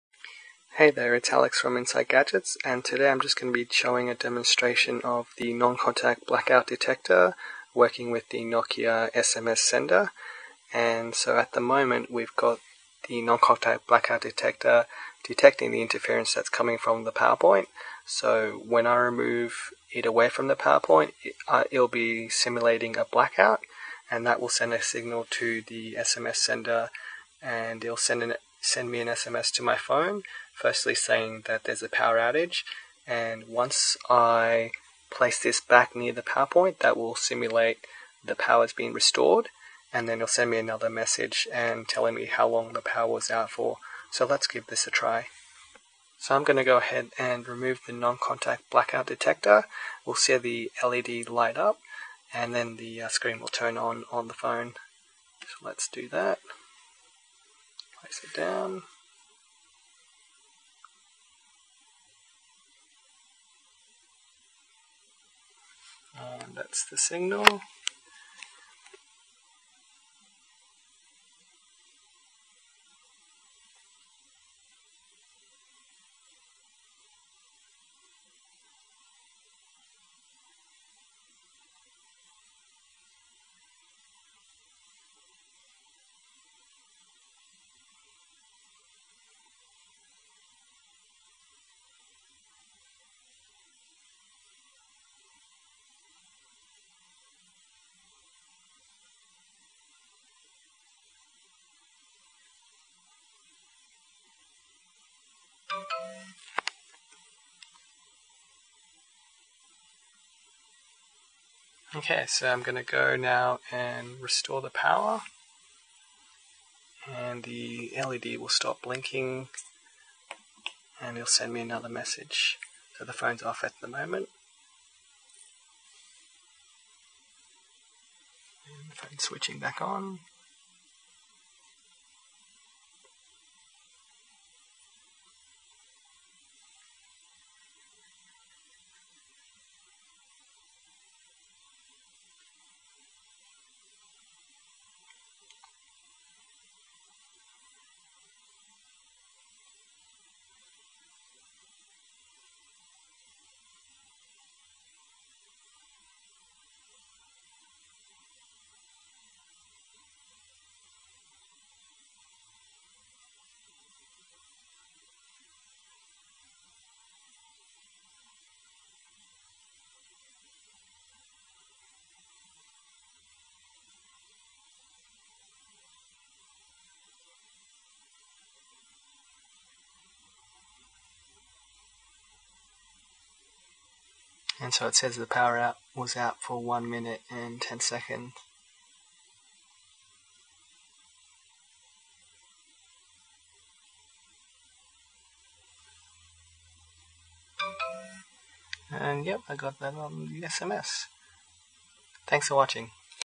Blackout SMS Sender Demonstration
This video shows two projects combined into one example project called the Blackout SMS Sender. It will SMS me once the power is out (simulated) and then will SMS me once power is restored with how long the power was out for.

The two projects are the Non-Contact Blackout Detector and Nokia 3120 Keypad SMS Sender.